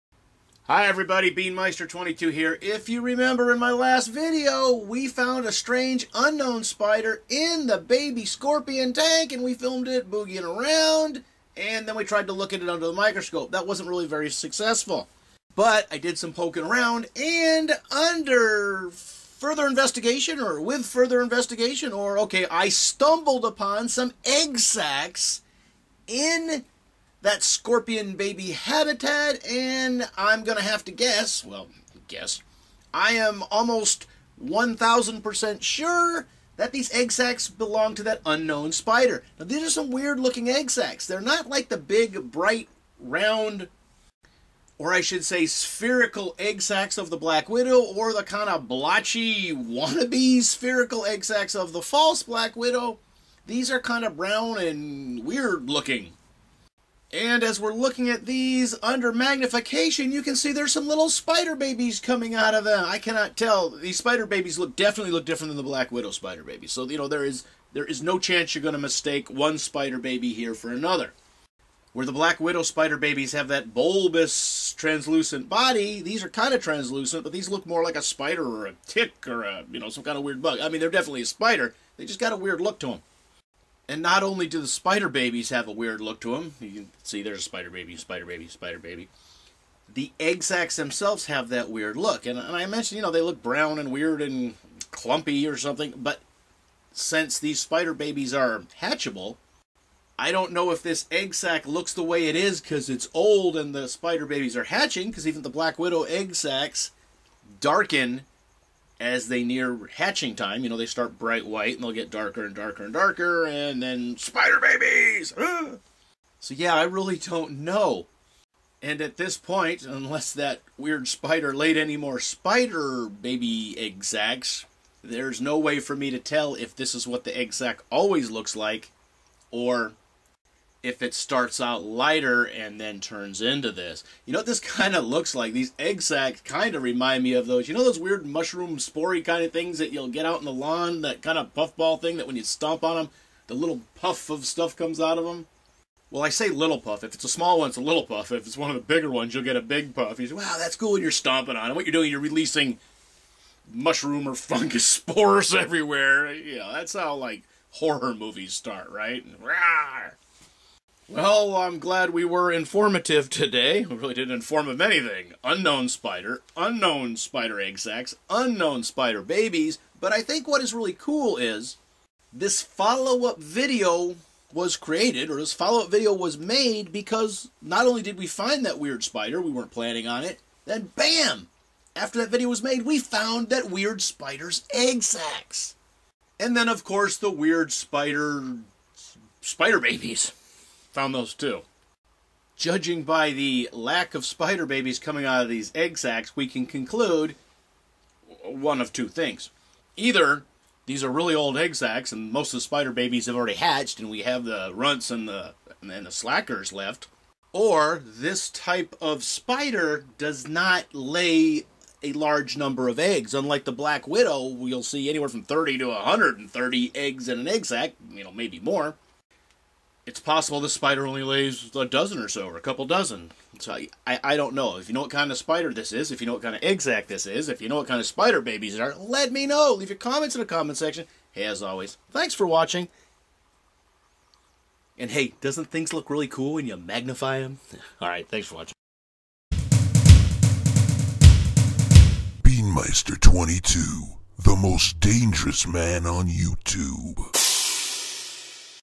Opening Spider Egg Sacks From Unknown Spider Found in Baby Scorpion Habitat
Can you identify this spider? If so, leave it in the comment section and help us out with this.
Desert Hairy Scorpion:
From Wikipedia: Hadrurus arizonensis, the giant desert hairy scorpion, giant hairy scorpion, or Arizona Desert hairy scorpion is the largest scorpion in North America, and one of the 8–9 species of Hadrurus in the United States, attaining a length of 14 cm (5.5 in). Its large size allows it to feed easily on other scorpions and a variety of other prey, including lizards and snakes. This species is usually yellow with a dark top and has crab-like pincers. It gets its common names from the brown hairs that cover its body. These hairs help it to detect vibration in the soil. A similar species is Hadrurus spadix